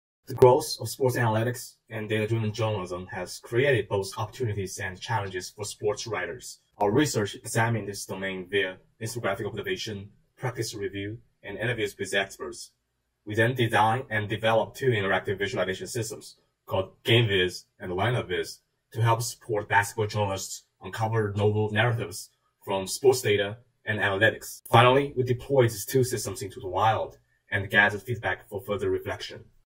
[Preview] Supporting Data-Driven Basketball Journalism through Interactive Visualization
Yu Fu, John Stasko

Session: Visualization Authoring & Creation

Abstract
Basketball writers and journalists report on the sport that millions of fans follow and love. However, the recent emergence of pervasive data about the sport and the growth of new forms of sports analytics is changing writers' jobs. While these writers seek to leverage the data and analytics to create engaging, data-driven stories, they typically lack the technical background to perform analytics or efficiently explore data. We investigated and analyzed the work and context of basketball writers, interviewed nine stakeholders to understand the challenges from a holistic view. Based on what we learned, we designed and constructed two interactive visualization systems that support rapid and in-depth sports data exploration and sense-making to enhance their articles and reporting. We deployed the systems during the recent NBA playoffs to gather initial feedback. This article describes the visualization design study we conducted, the resulting visualization systems, and what we learned to potentially help basketball writers in the future.

Video previews for CHI 2022 papers